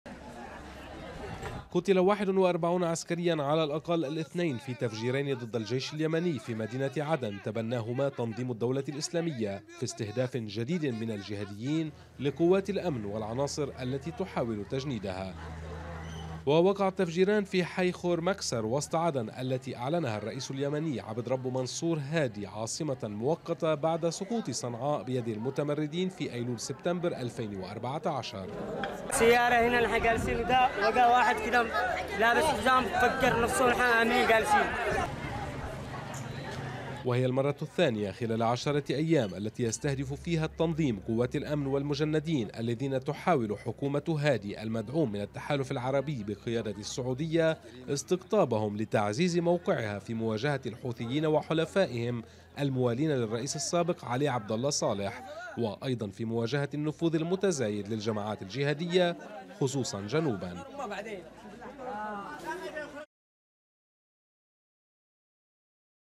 41 قتيلا في هجومين لتنظيم الدولة الاسلامية استهدفا الجيش اليمني
قتل 41 عسكريا على الاقل الاثنين في تفجيرين ضد الجيش اليمني في مدينة عدن (جنوب) وتبناهما تنظيم الدولة الاسلامية، في استهداف جديد من الجهاديين لقوات الامن والعناصر التي تحاول تجنيدها.تقرير مسجل: شربل باخوس.